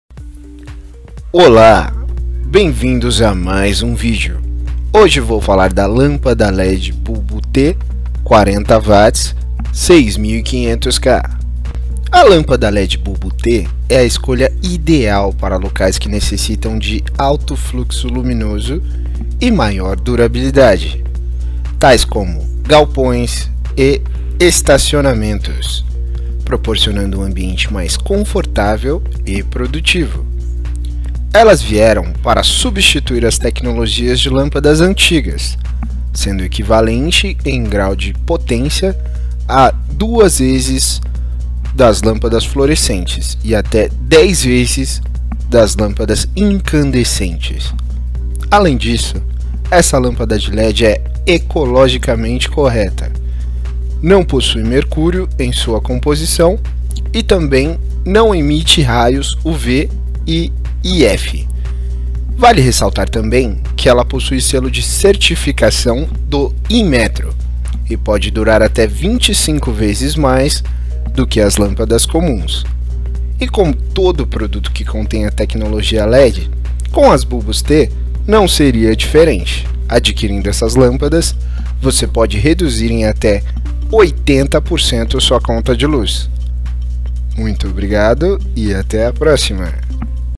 LÂMPADA LED BULBO T 40W 6500K
FICHA TECNICA
Potência: 40W
Base: E27
Tensão: 100~240V
Temperatura de Cor: 6500K
Ângulo de abertura: 180°
IRC: 80
Frequência Nominal: 50/60Hz
Fluxo luminoso: 3200lm
Fator Potência: 0,92
Corrente Nominal: 127V-310mA
                                  220V-175mA
Temp. de Operação: -10°C - -40°C
Peso Líquido: 367g
Vida Útil Nominal (L70): 25.000h